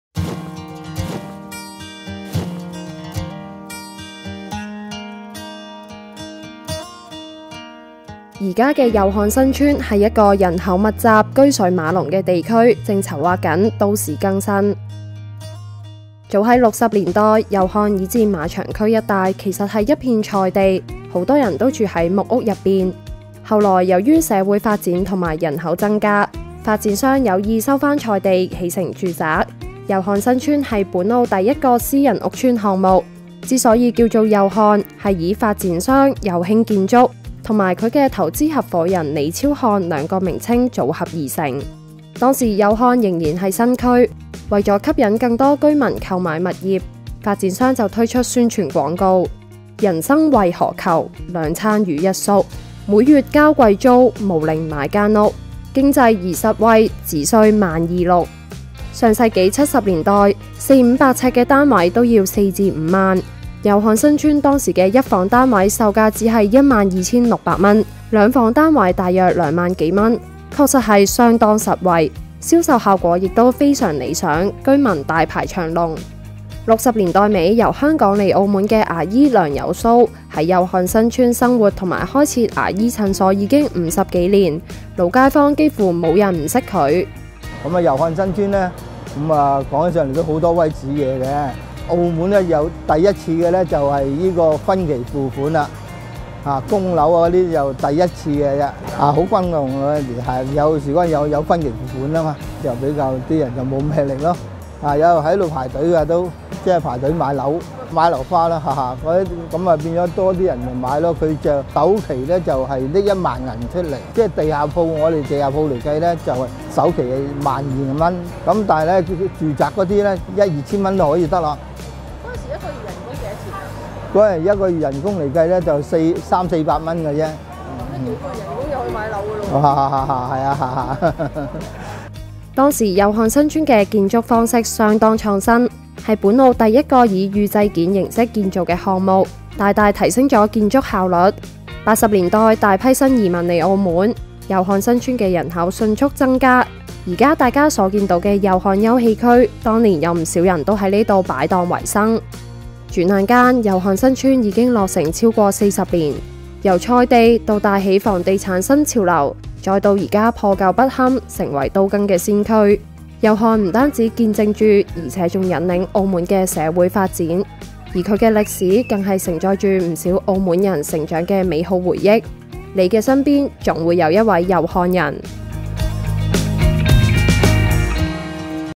走過祐漢新邨的足跡 | A Walk through Iao Hon Estate
祐漢新邨，是個人口密集、車水馬龍的地區，正籌劃都市更新。早在六十年代，祐漢以至馬場區一帶，其實是一片菜地，不少人都住在木屋.

A Walk through Iao Hon Estate

Iao Hon Estate is a densely populated and bustling area. It is also the first urban renewal project in Macau. As early as in the 1960s, the area between Iao Hon and Hipódromo used to be a vegetable field and many lived in wooden houses……